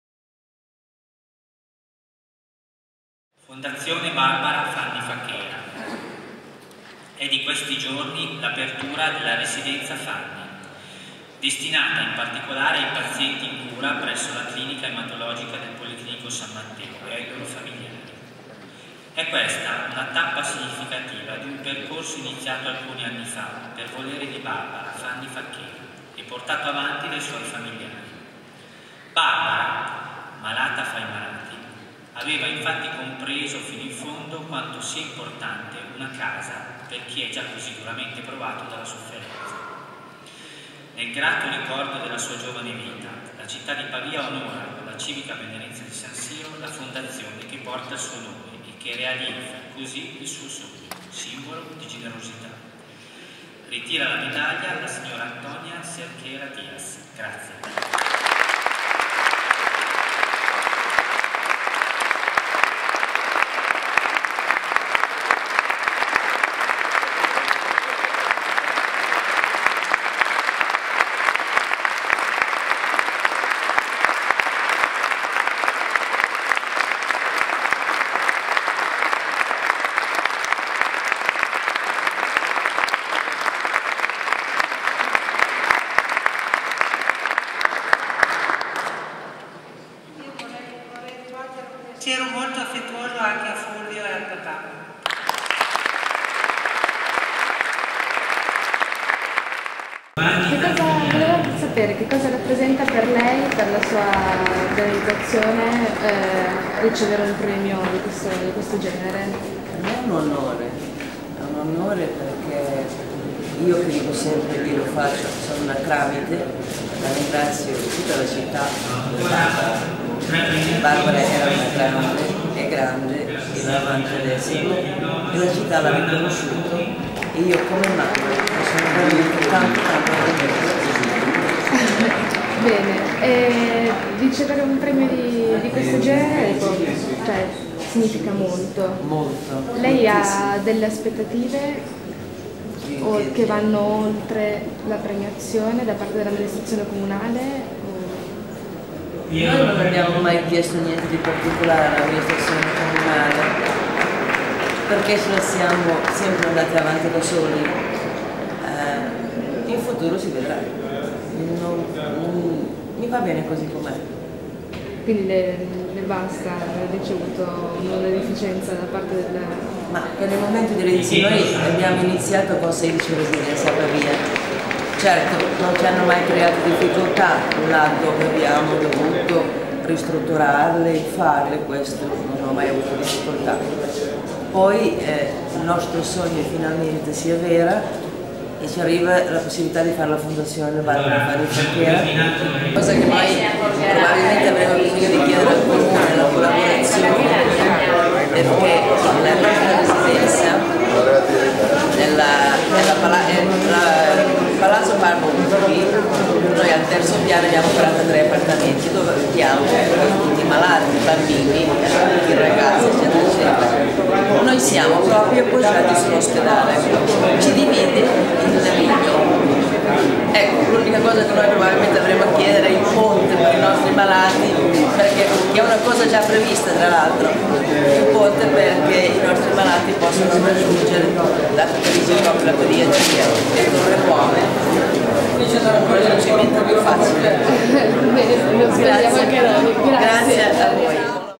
San Siro 2008 - Premiazione della "Fondazione Barbara Fanny Facchera"
Premiazione della "Fondazione Barbara Fanny Facchera", in occasione della cerimonia di conferimento delle Benemerenze Cittadine presso l'Aula Magna del Collegio Ghislieri di Pavia il giorno 9 dicembre 2008, giorno di San Siro, partono della città.

Intervista ada Antonia Cerqueira Dias.